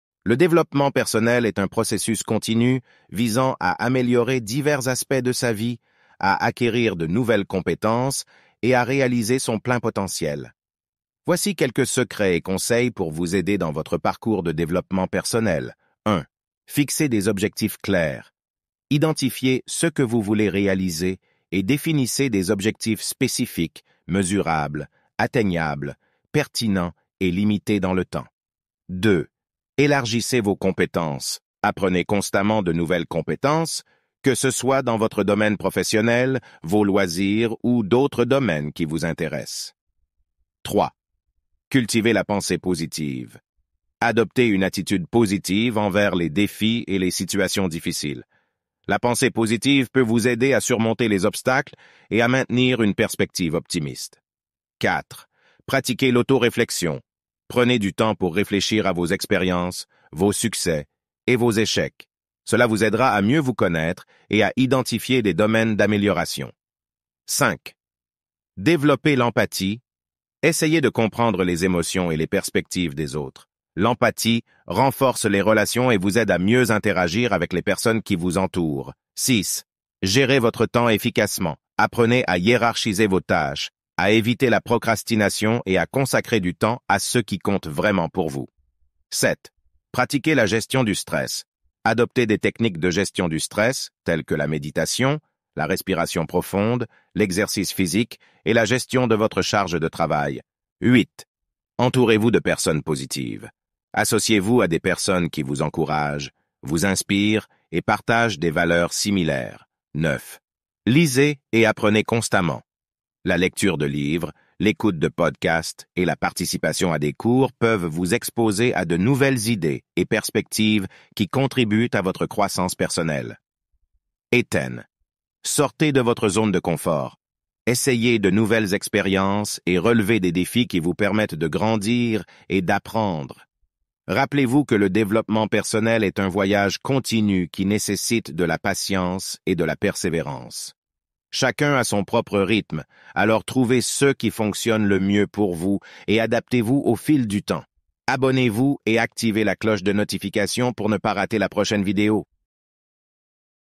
Dévelopment personnel : Quels sont les secrets pour se développer personnellement ?
Rejoignez-moi dans cette vidéo où je vous partage mes conseils et astuces pour apprendre à vous déveloper personnellement.